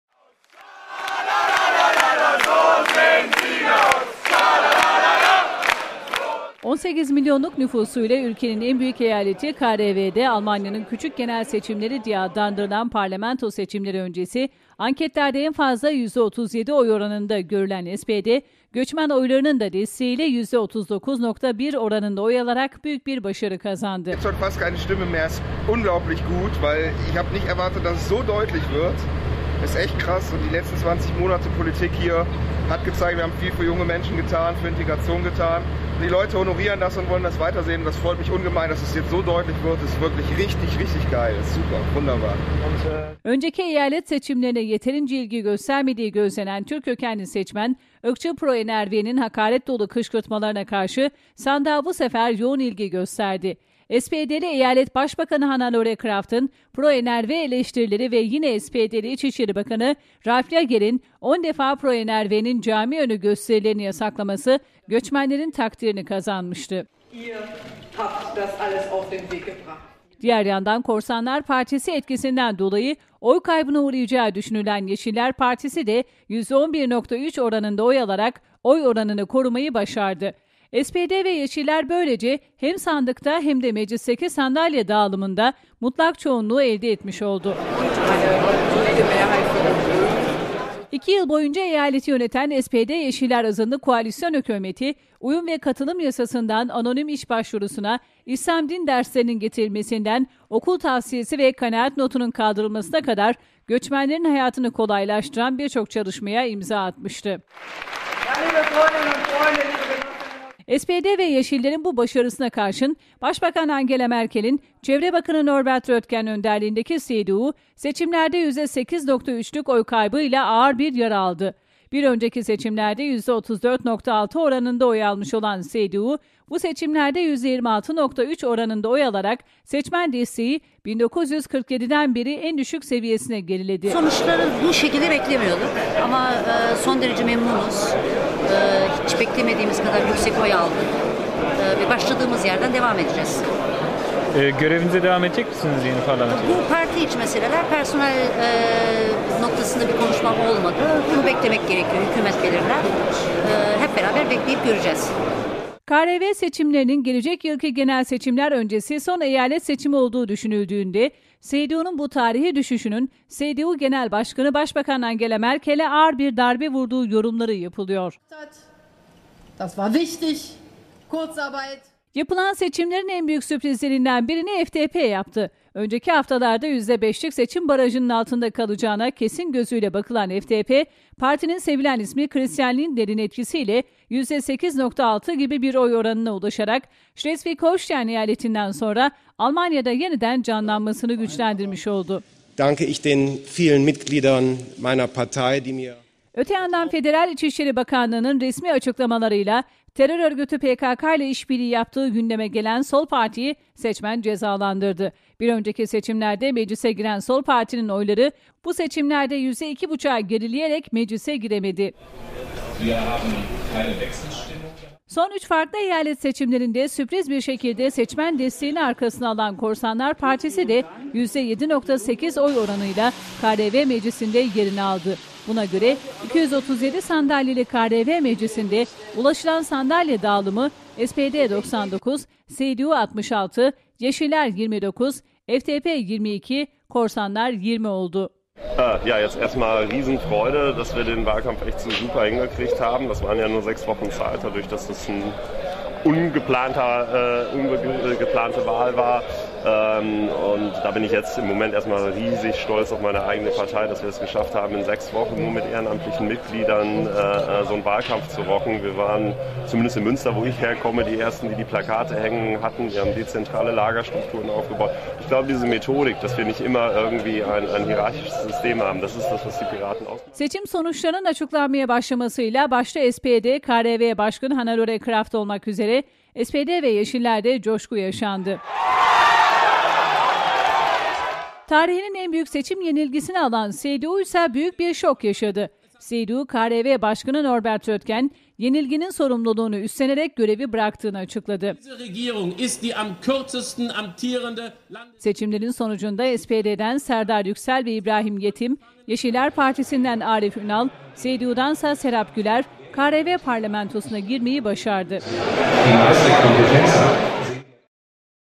MERKEL İÇİN SONUN BAŞLANGICI MI?
KRV'DE GÖÇMENLER SPD'Yİ UÇURDU, CDU İSE AĞIR YARA ALDI